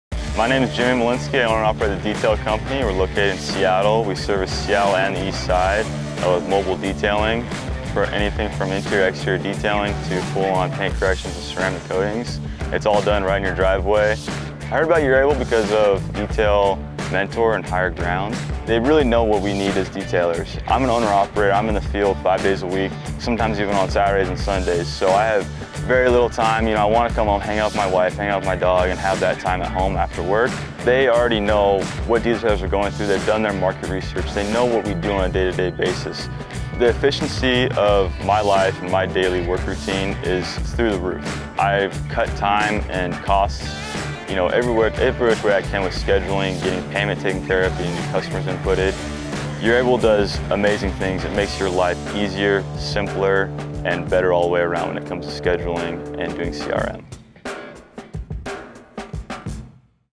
Jimmy Mulinski Is All About Efficiency | The Detail Company
Jimmy Mulinski discusses his mobile detailing business and the efficiencies he gains with Urable.

If you have questions, comments, or need assistance, we’re here to help via email or the live chat in Urable.